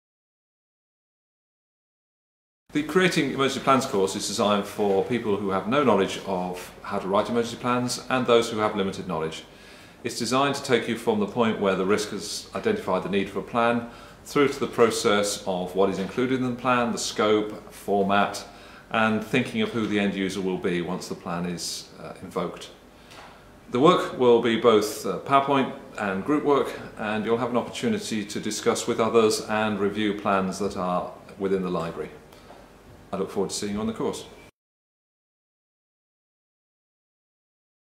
Creating Emergency Plans
By the end of this course you will be able to:

Describe the elements of the planning process
Develop planning templates for generic and specific arrangements
Evaluate your own organisation's plan development procedures